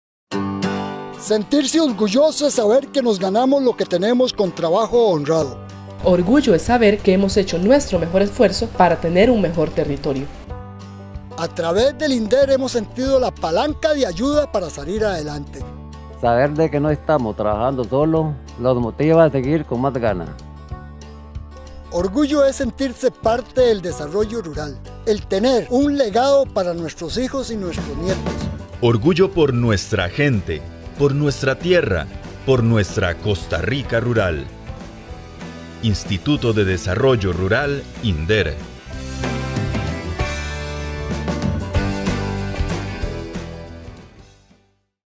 OrgulloRural por nuestra gente
Orgullo Rural es acompañar al pequeño productor para que pueda tener un trabajo digno, brindando las condiciones necesarias para desarrollar su actividad productiva de manera efectiva